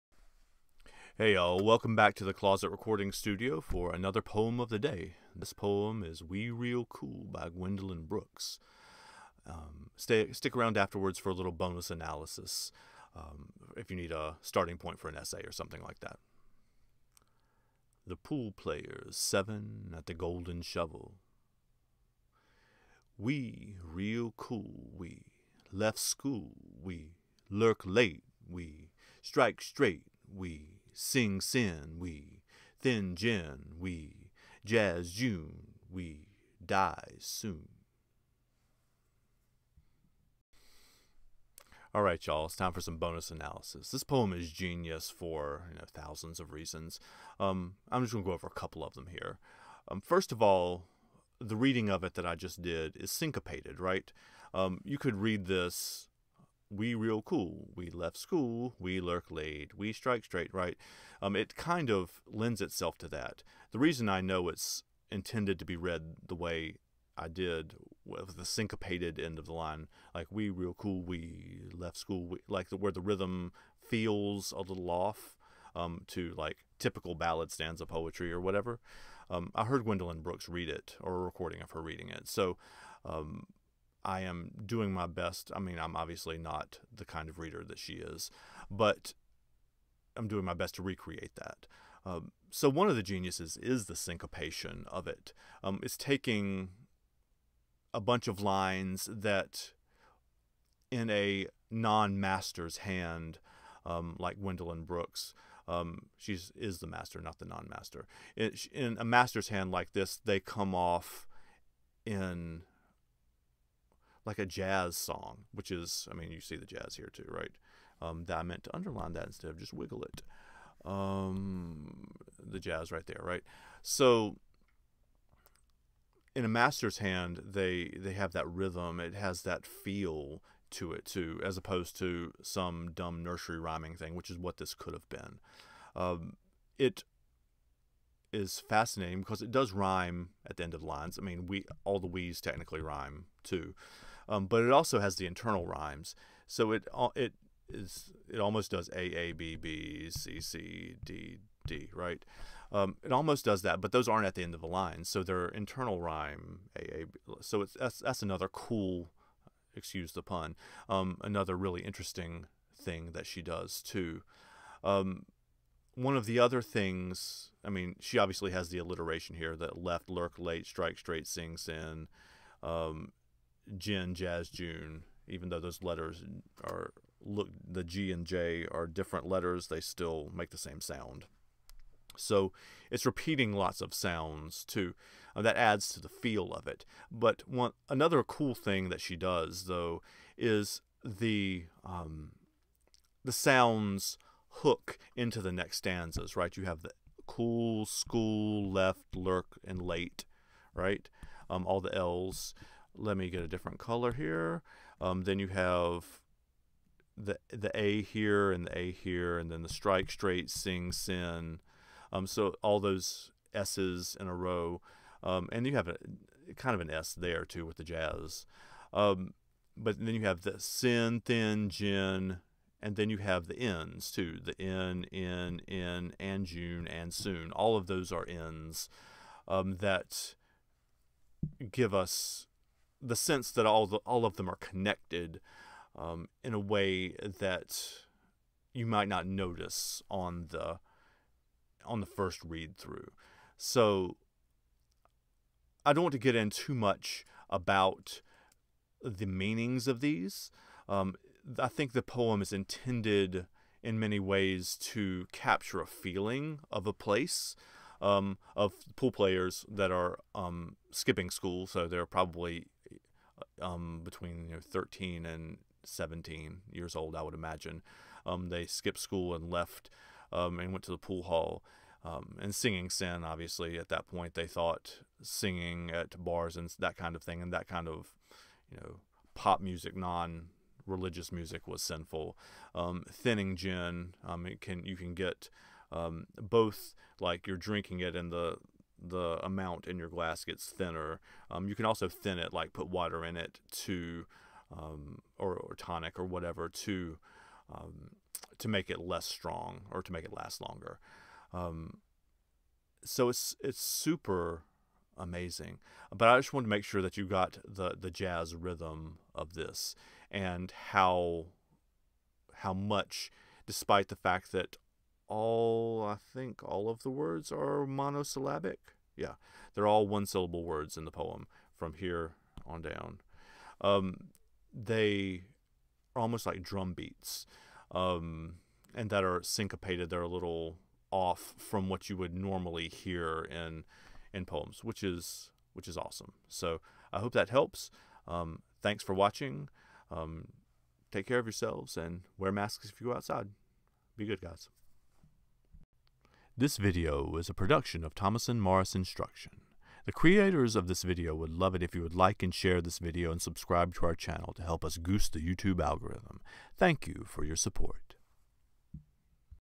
"We Real Cool" (Gwendolyn Brooks) - Poetry Read-Aloud and Bonus Analysis/Reaction
A read-aloud -- and then a veteran English teacher gives some commentary on structure, form, figurative language, etc. For the purposes of essay help!